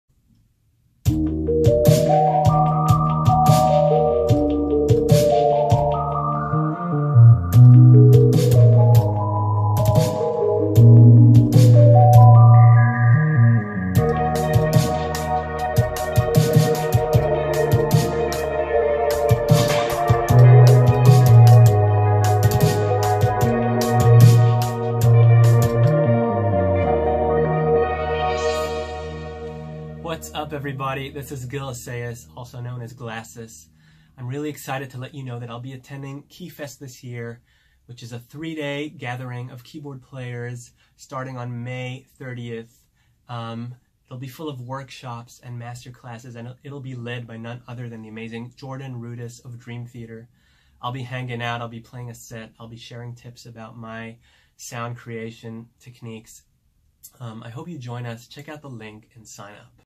Gil Assayas Joins Jordan Rudess's Keyfest 2019
Jordan Rudess of Dream Theater is bringing his KeyFest experience back to Sweetwater! With three days of jamming alongside, hanging out with, and learning from Rudess and guests David Rosenthal (Rainbow, Billy Joel, Cyndi Lauper) and Otmaro Ruíz (solo artist, John McLaughlin, Abraham Laboriel), Gil Assayas (Todd Rundgren's Utopia) KeyFest is an event no keys player should miss.